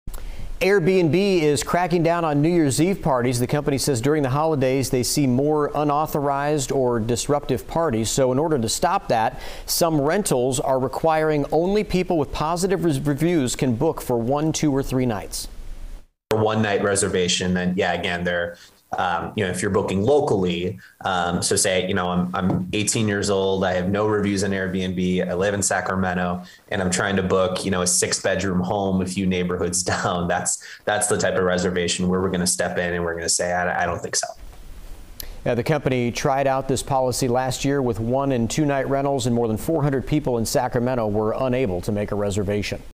'Protecting our hosts is the priority': Airbnb cracking down on Sacramento New Year's Eve parties
Airbnb is cracking down on chaotic New Year's Eve parties across the country and in Sacramento.  During the holidays, the rental company said it sees more unauthorized or disruptive parties. To try and stop parties, for some rentals, only people with positive reviews can book with the company.